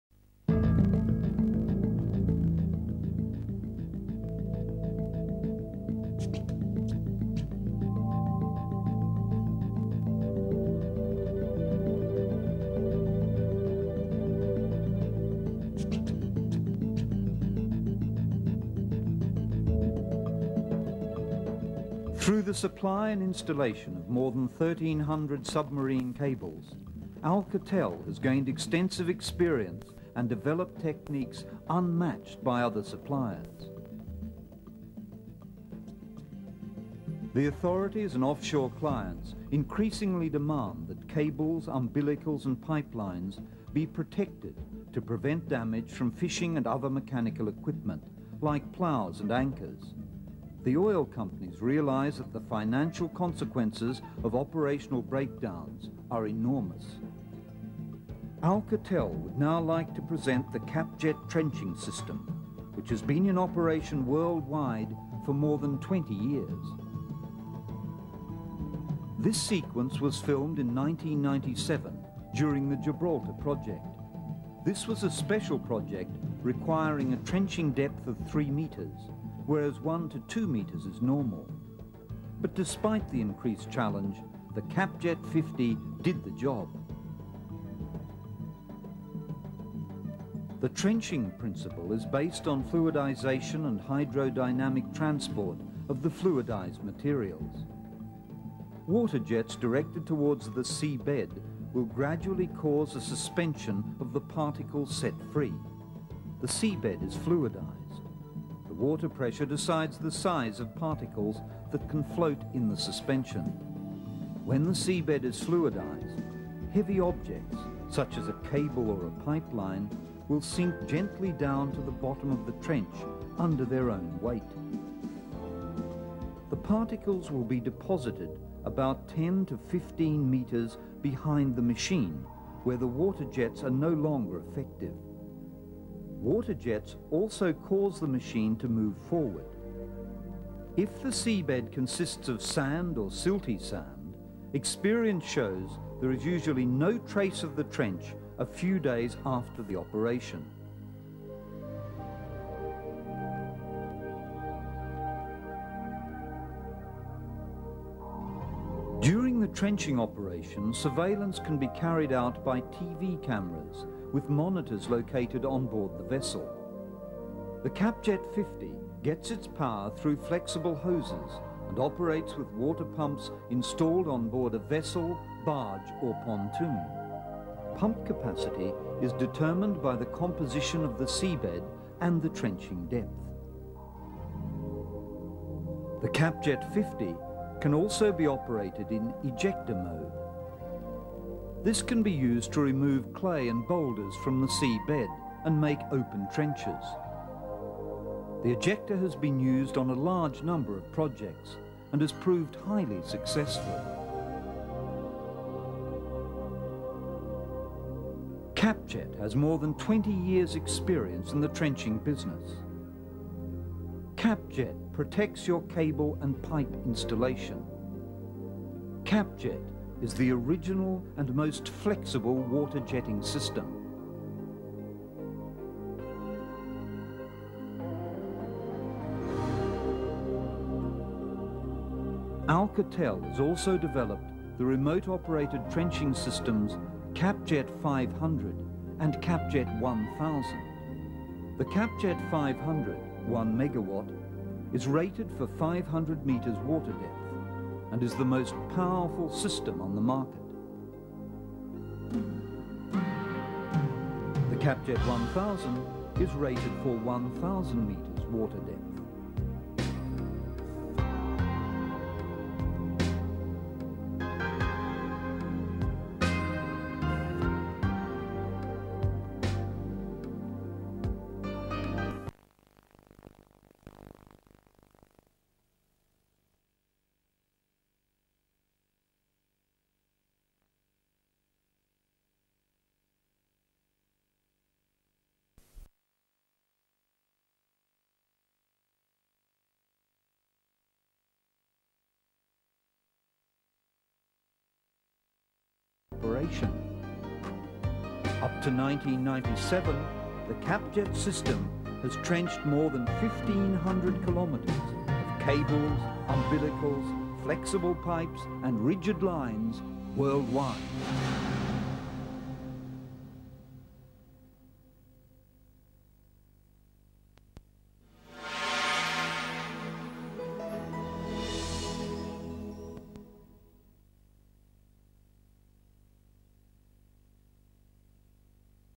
Alcatel - Capjet Trenching System - 1997
Capjet 50 - 20 years of experience worldwide operation ..

VHS tilhørende Alcatel \ Standard Mekaniske fra 1997